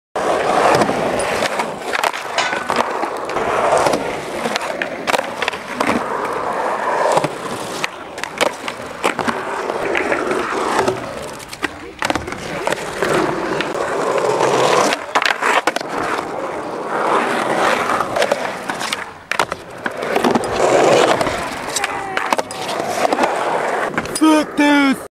kevin skating på sportsplassen
noen trix på sportsplassen..